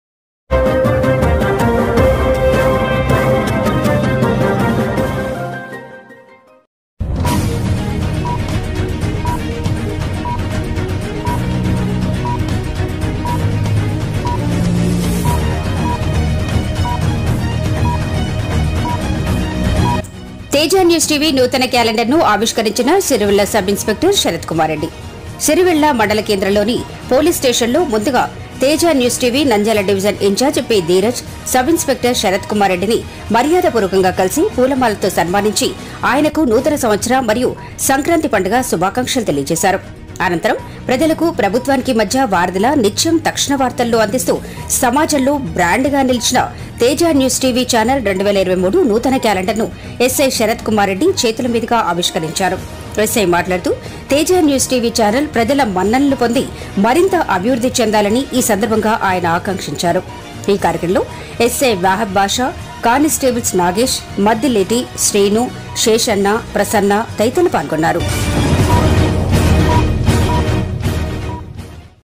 Teja News TV నూతన క్యాలెండర్ ను ఆవిష్కరించిన శిరివెళ్ళ సబ్ ఇన్స్పెక్టర్ శరత్ కుమార్ రెడ్డి
_శిరివెళ్ళ మండల కేంద్రము లోని పోలీస్ స్టేషన్ లో ముందుగా_   *తేజా న్యూస్ టీవీ నంద్యాల డివిజన్ ఇంఛార్జి  పి.ధీరజ్ సబ్ ఇన్స్పెక్టర్ శరత్ కుమార్ రెడ్డి*  _గారిని మర్యాద పూర్వకంగా కలిసి *పూల మాలతో* సన్మానించి ఆయనకు *నూతన సంవత్సర మరియు సంక్రాంతి పండుగ* శుభాకాంక్షలు తెలియజేశారు._

        _అనంతరం ప్రజలకు ప్రభుత్వానికి మధ్య వారదిలా నిత్యం తక్షణ వార్తలను అందిస్తూ సమాజంలో బ్రాండుగా నిలిచి న తేజా న్యూస్ టీవీ ఛానల్  2023 నూతన క్యాలెండర్ ను ఎస్.ఐ శరత్ కుమార్ రెడ్డి గారి చేతుల మీదుగా ఆవిష్కరించారు._

       _ఎస్.ఐ మాట్లాడుతూ తేజా న్యూస్ టీవీ ఛానల్ ప్రజల మన్ననలు పొంది మరింత అభివృద్ధి చెందాలనీ ఈ సందర్భంగా ఆయన ఆకాంక్షించారు._

        _ఈ కార్యక్రమంలో ఏ.ఎస్.ఐ వహాబ్ బాషా , కానిస్టేబుల్స్ నాగేష్ , మద్దిలేటి , శ్రీను , శేశన్న , ప్రసన్న , తదితరులు  పాల్గొన్నారు._